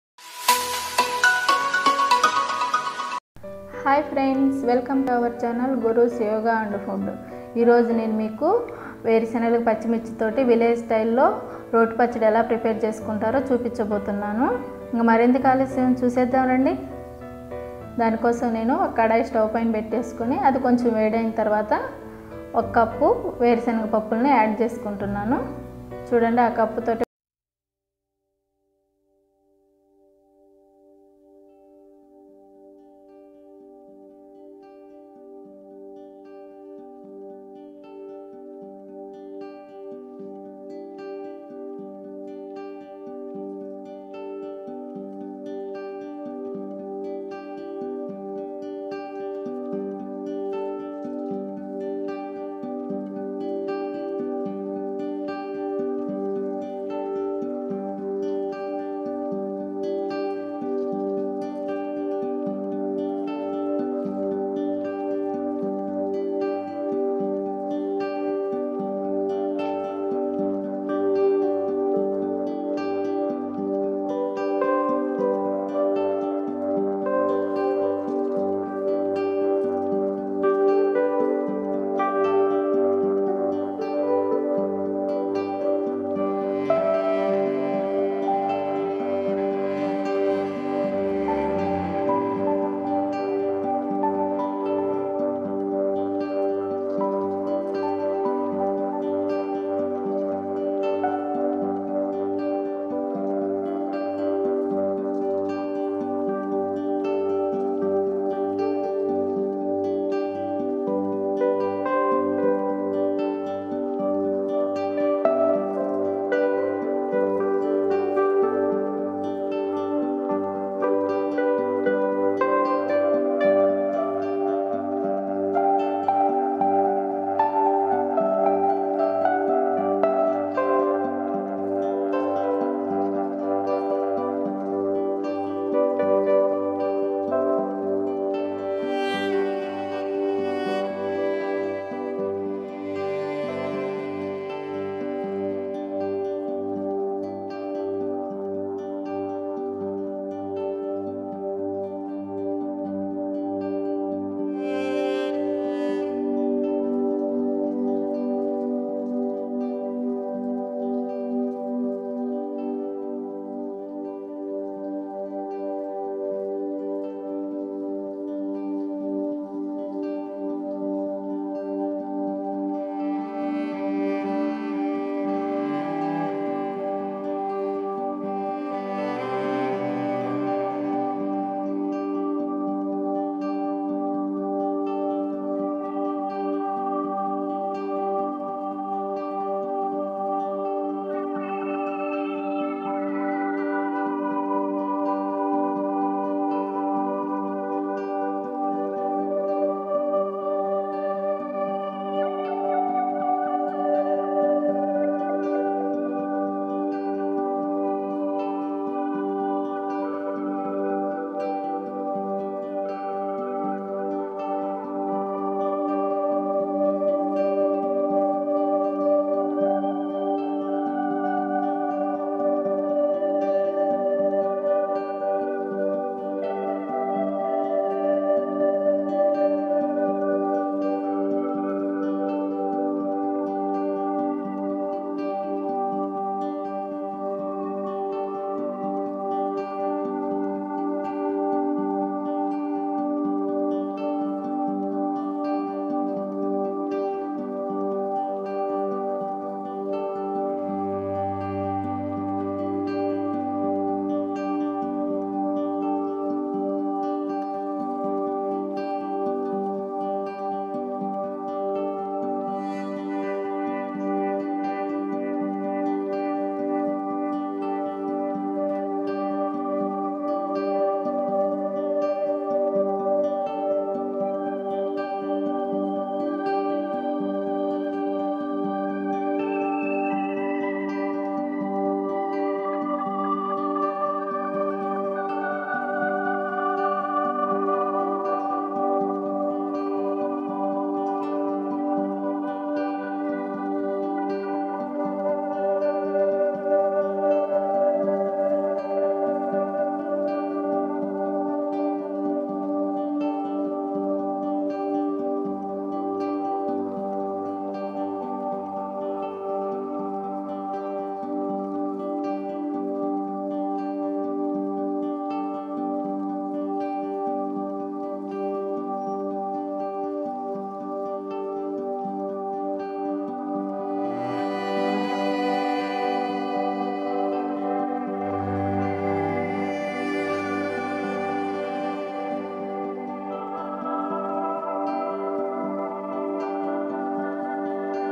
Palli Chutney || Peanut Chutney Recipe In Telugu || How to make Chutney
పచ్చిమిర్చి,వేరు శనగలతో రోటి పచ్చడి||Preparation of Green Chilli, Peanuts Chutney|| Guru's YOGA and FOOD.